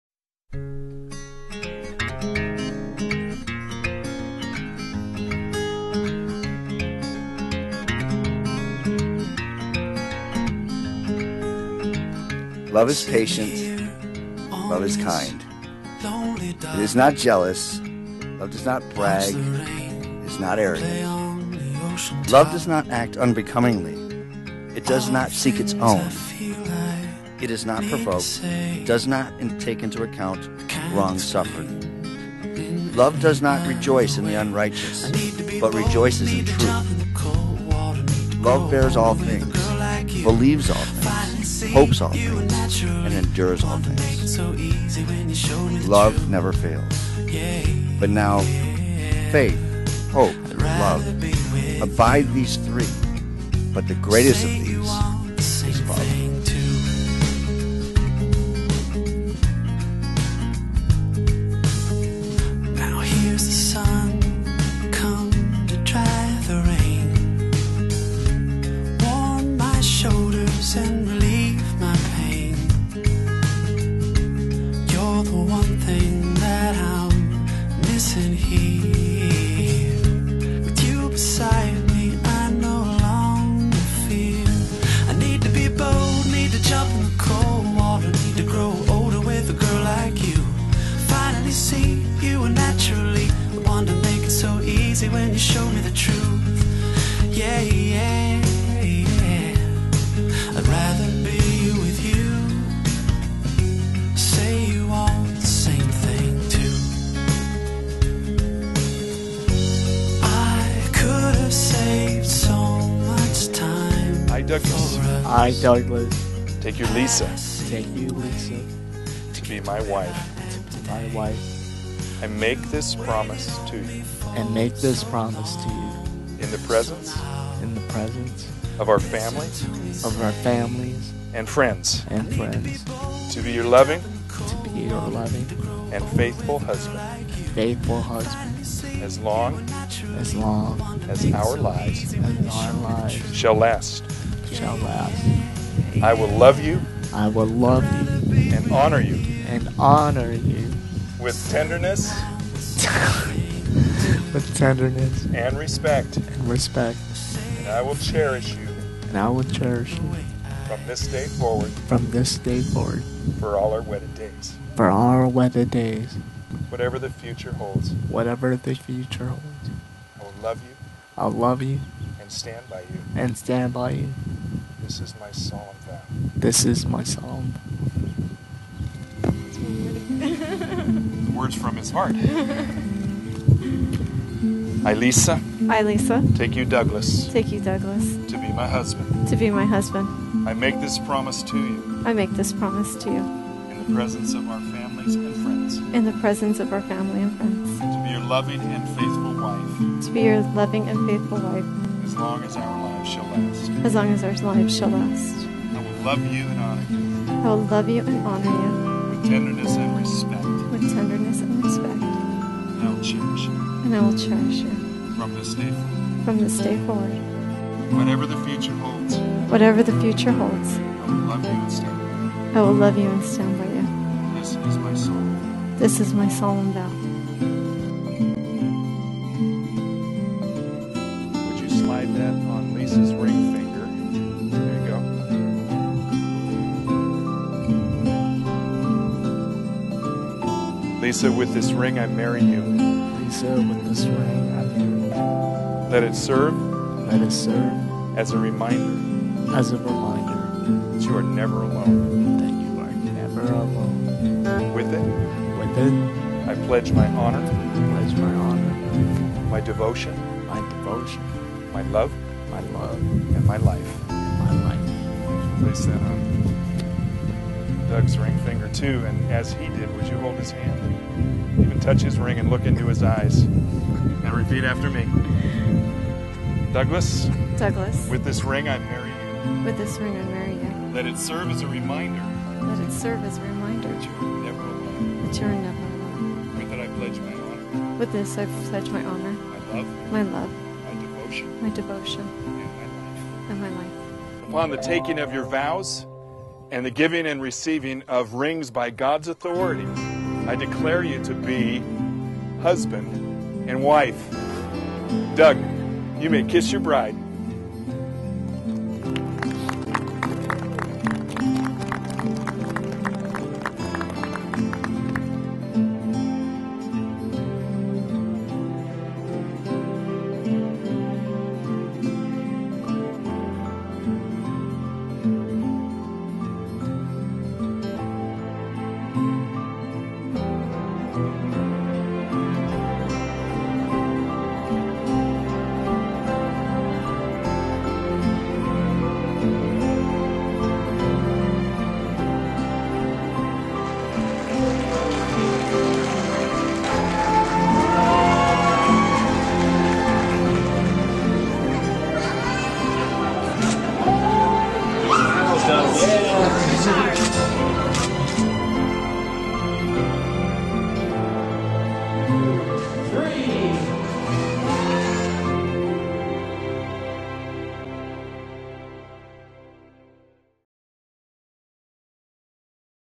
The Wedding of Lisa & Doug - Wish Upon A Wedding (Southern California)
This is the highlights of the latest wish recipients of Wish Upon a Wedding southern California. Lisa and Doug were married on 5/26/2011. This video was produced for Wish Upon a Wedding by New Classic FIlms. for more information.